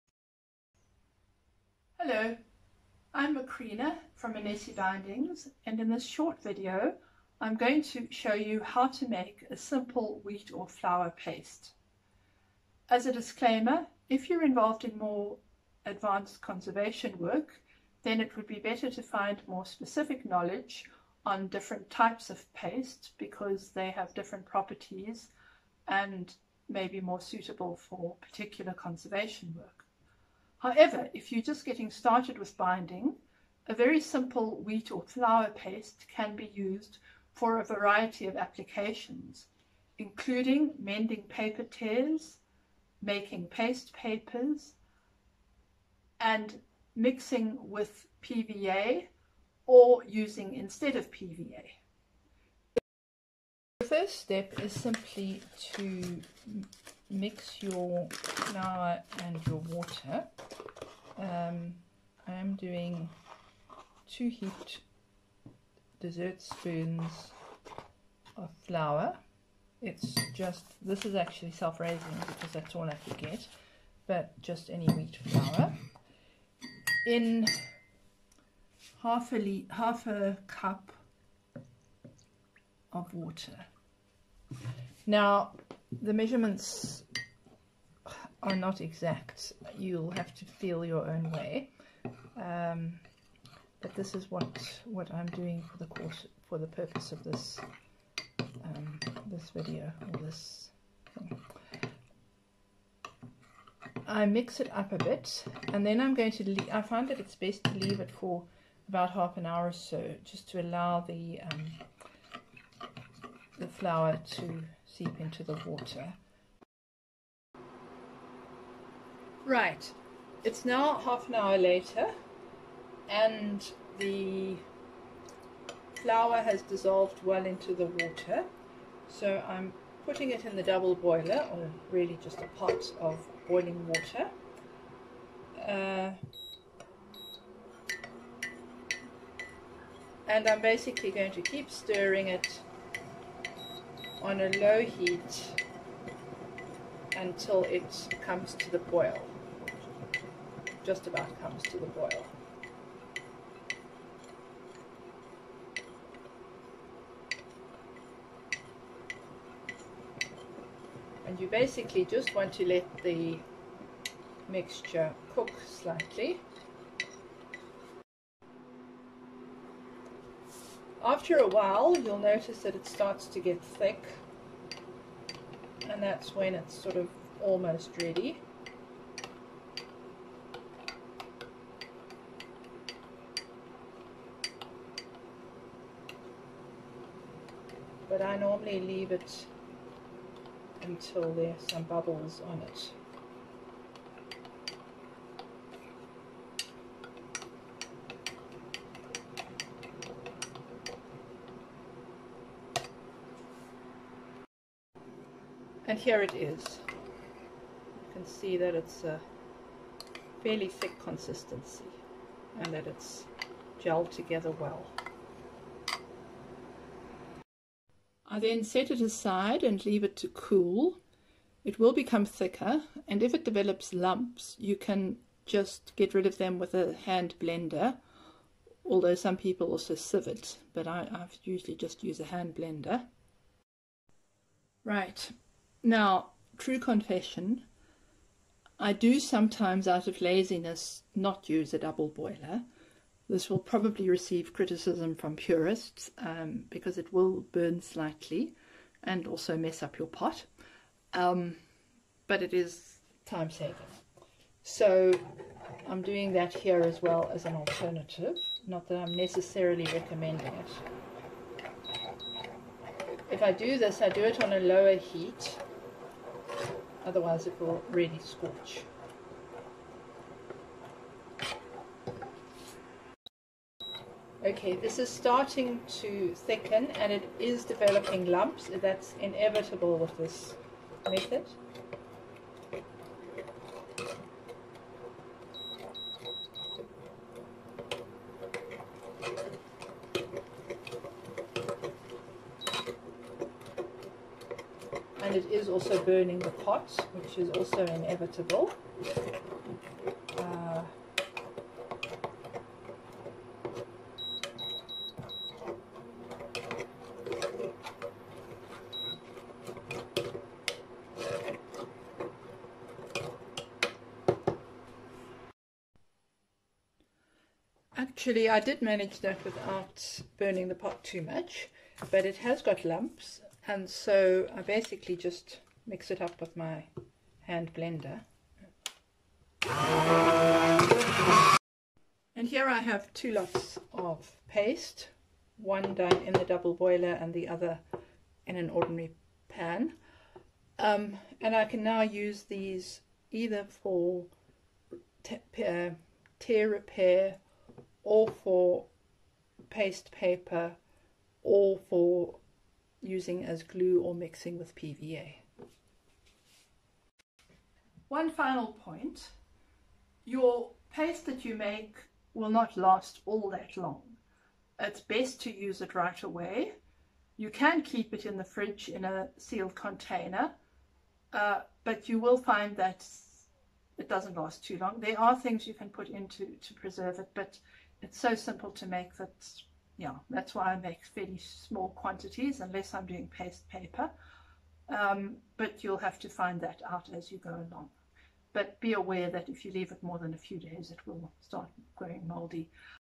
Making Bookbinding Paste
In this video, I show how to make a simple wheat or flour paste that can be used for various bookbinding applications.